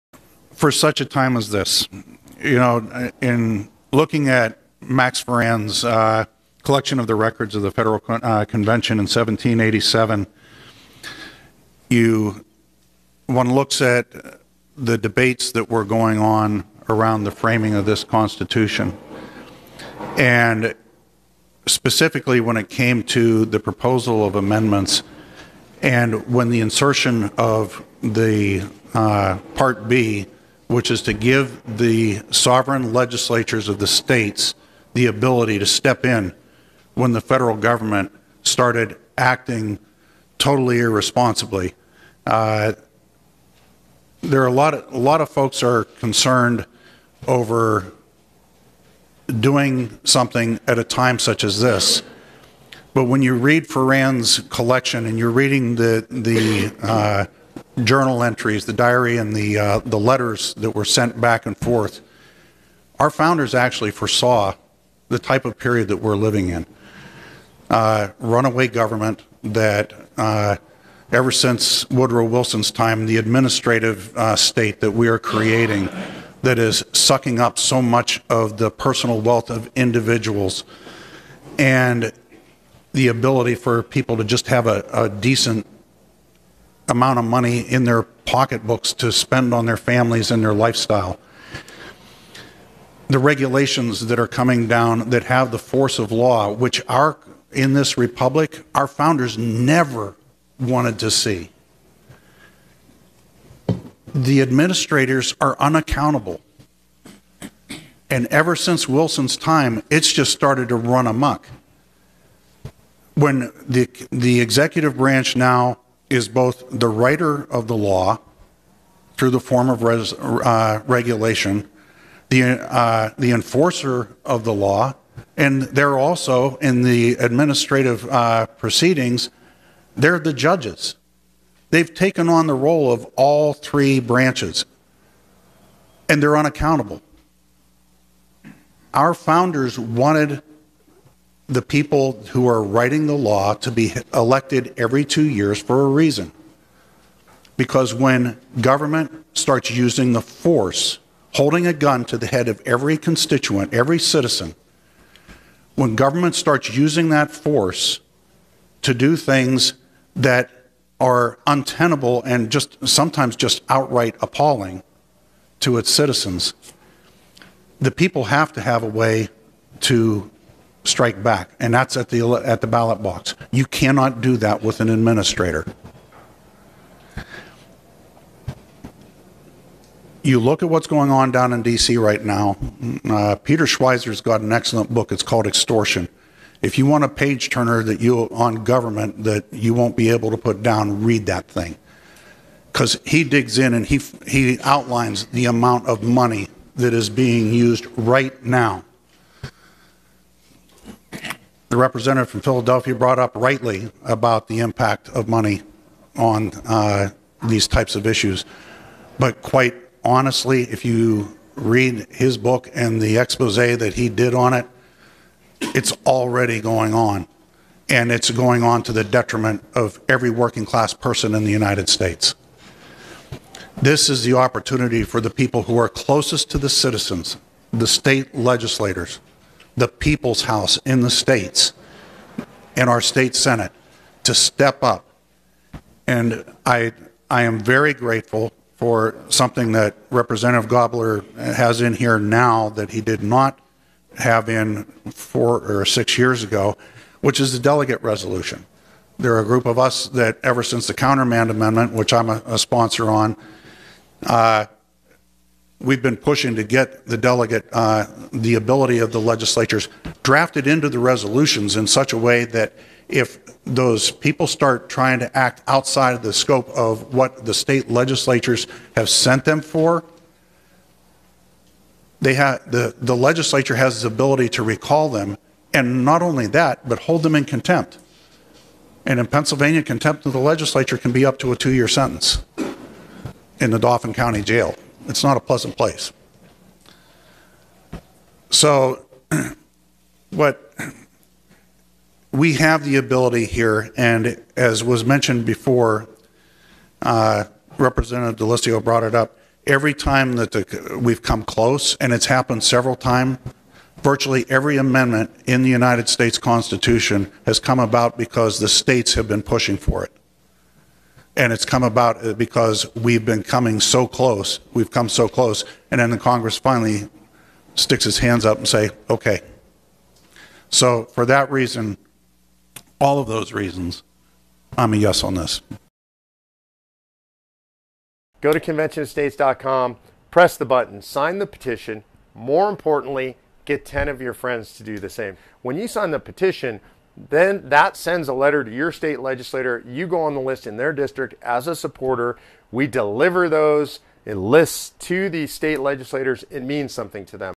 Pennsylvania Rep. Cris Dush: the Founders never wanted unaccountable bureaucrats writing the laws
State Representative Cris Dush explains why he voted yes for Convention of States.